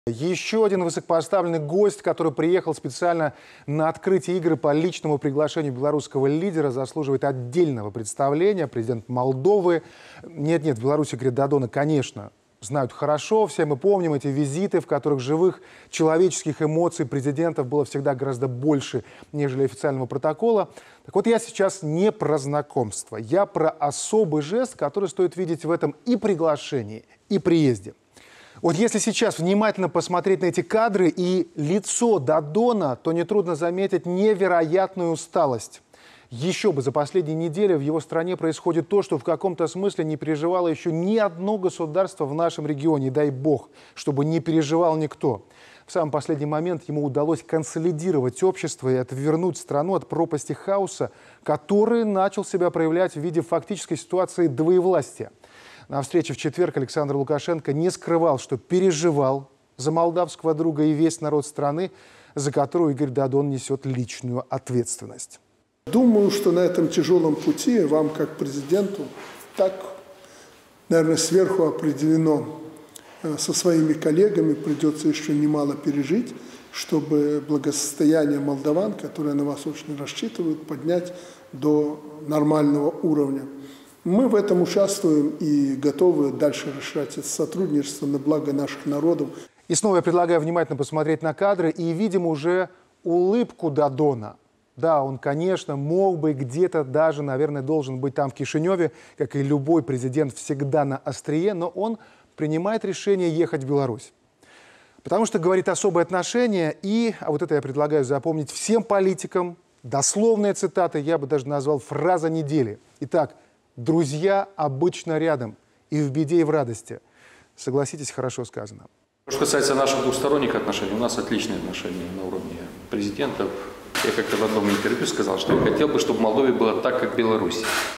Игорь Додон: Я хотел бы, чтобы в Молдове было так, как в Беларуси
Ещё один высокопоставленный гость, который приехал специально на открытие Игр по личному приглашению белорусского лидера заслуживает отдельного представления. Президент Молдовы. в Беларуси Игоря Додона, конечно, знают хорошо. Все мы помним эти визиты, в которых живых человеческих эмоций президентов было всегда гораздо больше,  нежели официального протокола. Но сейчас речь идёт про особый жест, который стоит видеть в этом и приглашении, и приезде.